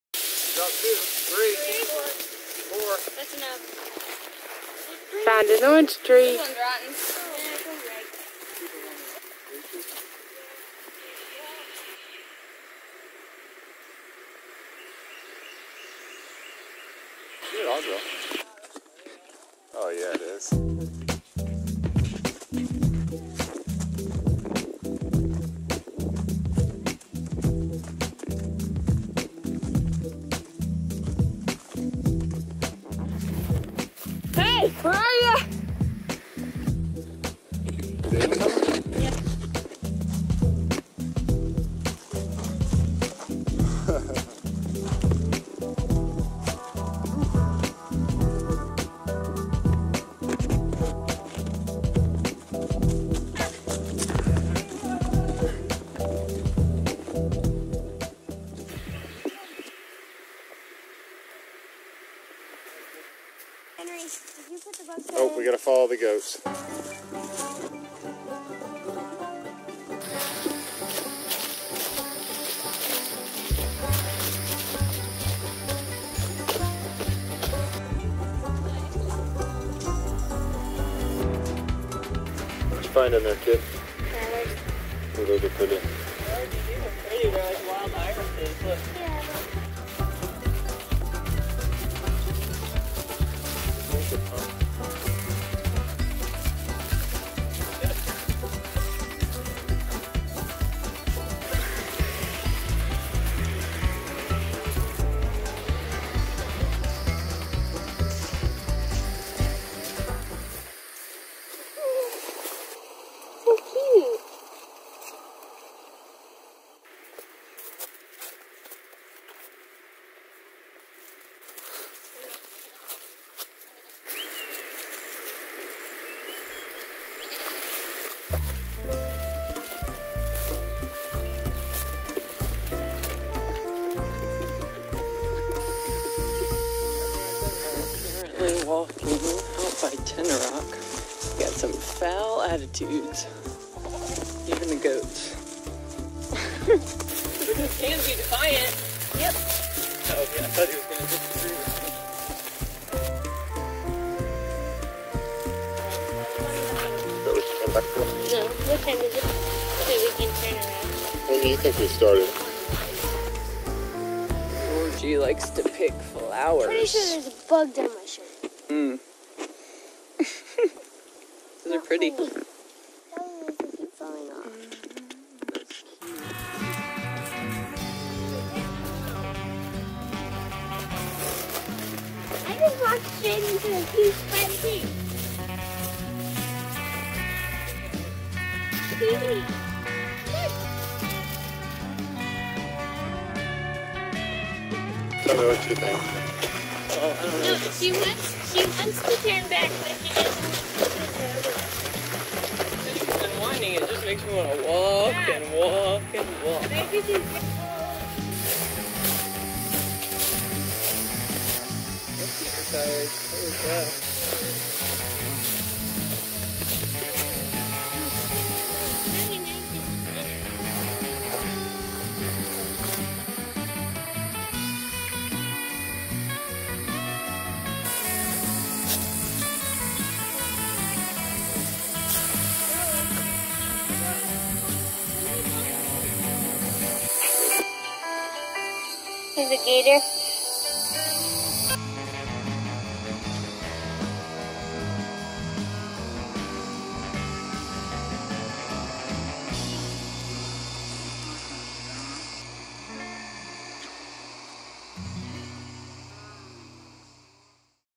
Saddle Creek and Tenoroc Trails
A fun find of a trail that we've driven by for years. These trails offer a lot of variety, and follow the creek between Saddle Creek and Tenoroc.

They are maintained by the Florida Trail Association, and require a $3 permit.